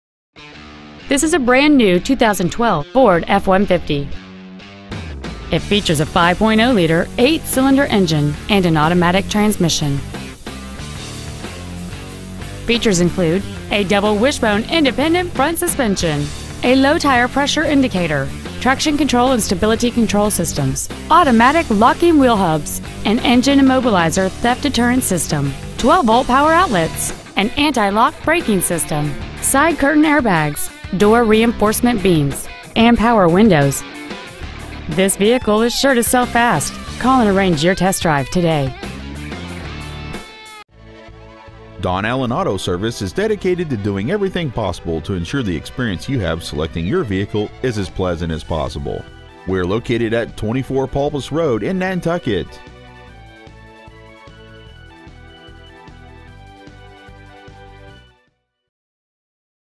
2012 Ford F-150 Nantucket MA
We've been honored to serve the Nantucket MA area , we promise that your experience at our dealership will exceed your expectations !

 Year : 2012
 Make : Ford
 Model : F-150
 Engine : 5.0L V8 32V MPFI DOHC FLEXIBLE FUEL
 Trans . : 6-SPEED AUTOMATIC
 Exterior : RED
 Miles : 10

 Don Allen Auto Service Inc.